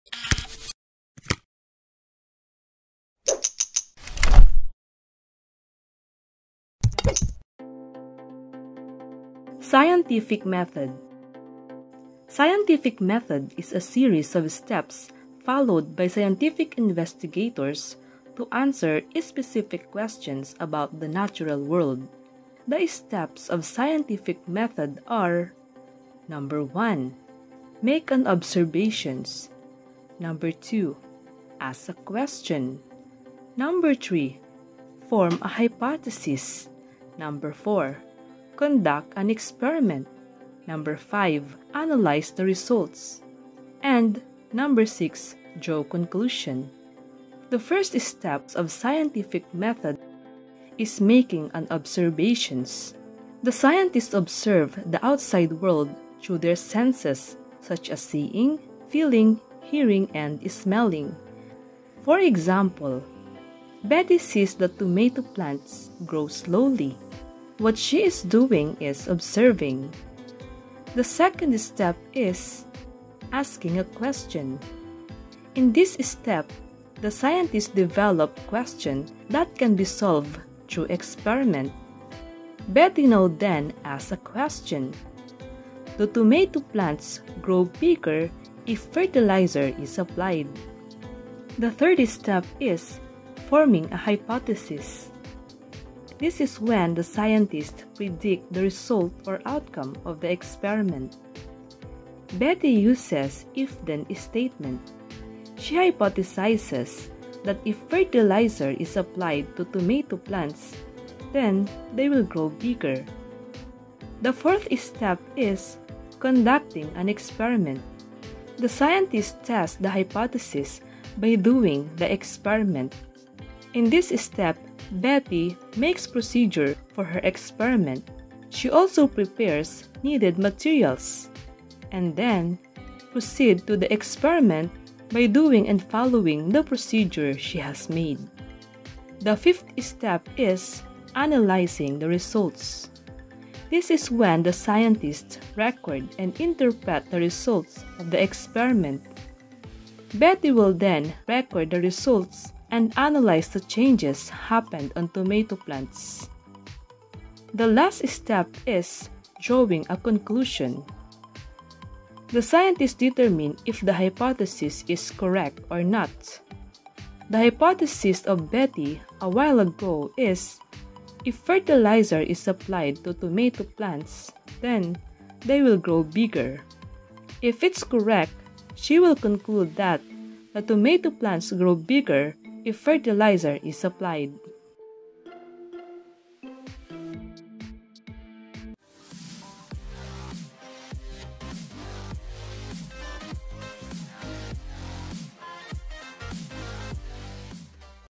Scientific Method I Terms and Examples I Simple Explanation
SCIENTIFIC METHOD

Scientific Method is a series of steps followed by scientific investigators to answer specific questions about the natural world.

The Steps of Scientific Method
1. Make an Observations
2. Ask a Question
3. Form a Hypothesis
4. Conduct an Experiment
5. Analyze the Results
6.  Draw Conclusion

SHORT EXPLANATION:
1. Making an Observations
The scientists observe the outside world through their senses such as seeing, feeling, hearing, and smelling.
For example: Betty sees tomato plants grow slowly.

2. Asking a Question
The scientists develop question that can be solved through experiment.
For example: Betty asks a question, "do plants grow bigger if fertilizer is applied?"

3. Forming a Hypothesis
The scientists predict the result or outcome of the experiment.
Hypothesis is an educated guess about the relationship between the independent and controlled variables.
For example: Betty hypothesizes that If fertilizer is applied to tomato plants, then they will grow bigger.

4. Conducting an Experiment.
The scientists Test the hypothesis by doing the experiment.
For example: Betty makes procedure for her experiment. She also prepares needed materials and then, proceed to the experiment by doing and following the procedures she has made.

5. Analyzing the results.
The scientists record and interpret the results of the experiment.
For example: Betty records the results and analyzes the changes happened on tomato plants.

6. Drawing conclusion.
The scientists determine if the hypothesis is correct or not.

The hypothesis of Betty, a while ago, is If fertilizer is applied to tomato plants, then they will grow bigger.

If it’s correct, she will conclude that the Tomato Plants grow bigger if fertilizer is applied.